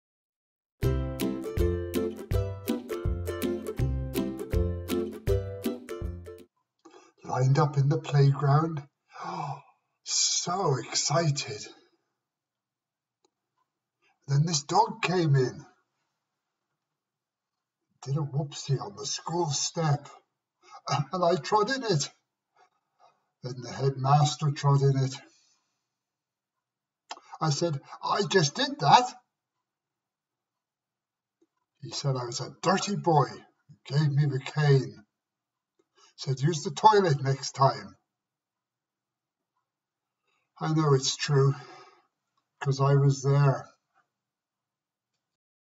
Given the cane on my first day in School
Just 5 years old, my first day, its all so unfair, just a misunderstanding.